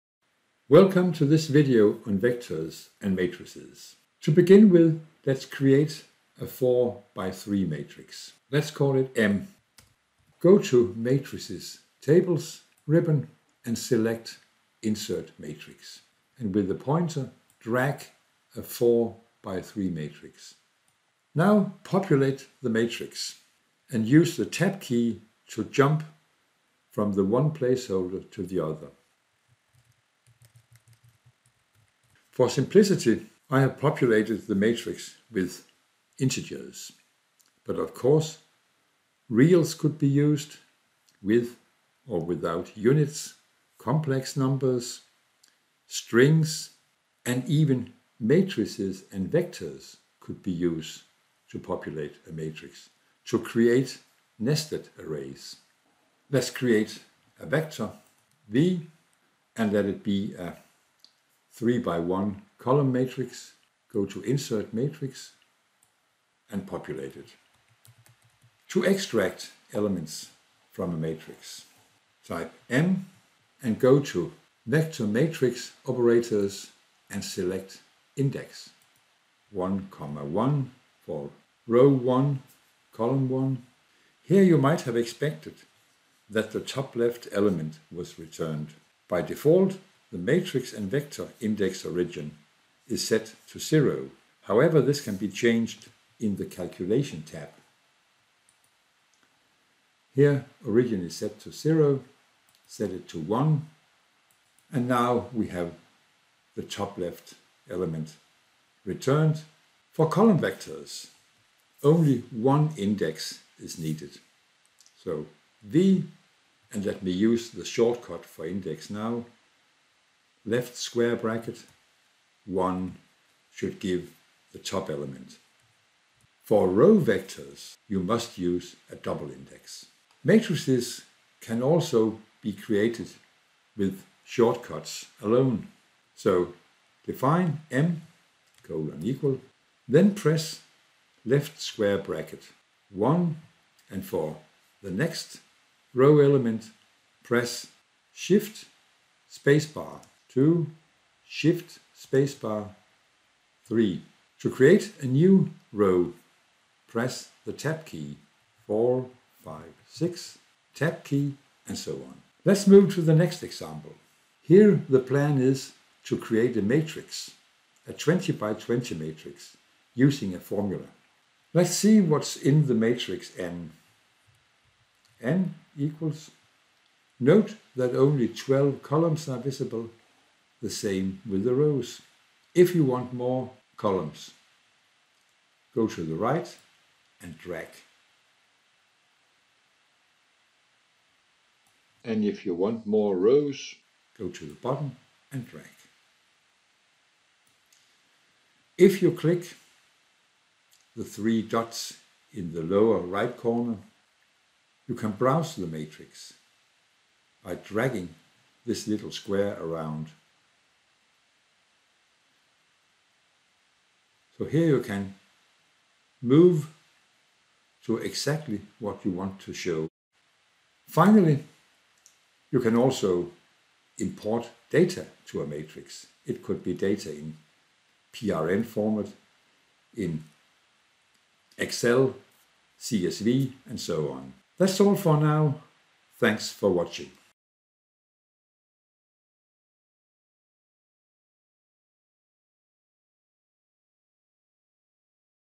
Basics of using Vectors and Matrices in Mathcad Prime
Defining and populating matrices and vectors in PTC Mathcad Prime is very easy and flexible. You can use the ribbon to create a template for a matrix or use keyboard shortcuts to build and populate a matrix in one go. Matrices can also be created using a formula and be used as a container for imported data.
This video shows you:
• How to define a vector and a matrix in Mathcad Prime
• How to extract elements from a vector or a matrix
• How to set your ORIGIN, or the index value of the first element
• How to define a vector and a matrix using keyboard shortcuts
• How to define a matrix using a formula (or imported data)
• How to display more columns/rows, or freely browse through the data, in large matrices

You can start incorporating vectors and matrices into your own work by downloading Mathcad Prime . Vectors and matrices are included in the free version, Mathcad Express, as well.